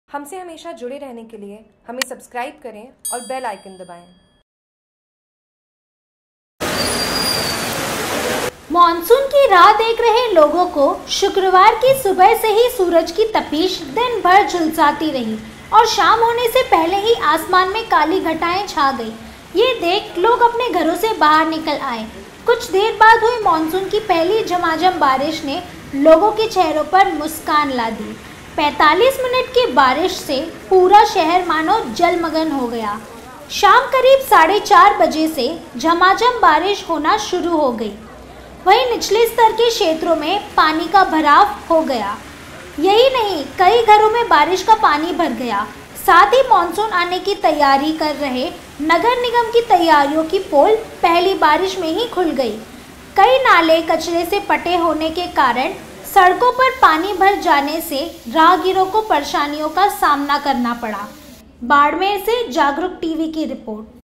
JAGRUK TV: बाड़मेर-मानसून की पहली अच्छी झमाझम बारिश से लोगों के चेहरे खिले
मानसून की राह देख रहे लोगों को शुक्रवार की सुबह से ही सूरज की तपिश दिन भर झुलसाती रही और शाम होने से पहले ही आसमान में काली घटायें छा गई।